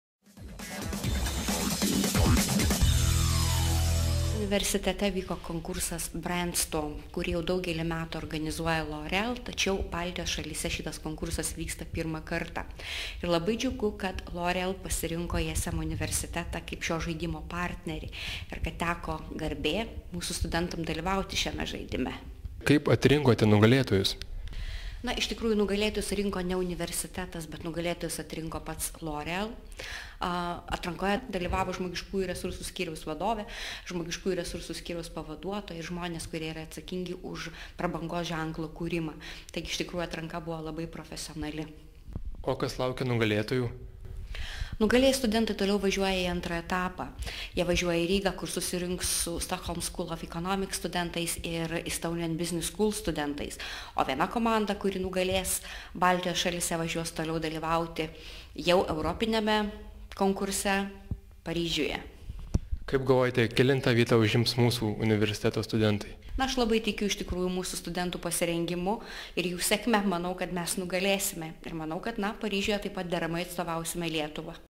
"L'Oreal Brandstorm 2010" konkursas ISM universitete
Kovo 22 dieną ISM universitete vyko atranka į L'Oreall rinkodaros konkursą Brandstorm 2010. Trys ISM studentų komandos, nugalėtojos, keliaus į Rygą susirungti su SSE Riga bei Estonian Business School komandomis. Latvijos sostinėje laimėjusi komanda keliaus į L'Oreal rinkodaros konkurso finalą Paryžiuje.
 ISM  vienintelė aukštoji mokykla Lietuvoje, kuri buvo pakviesta dalyvauti šiame konkurse.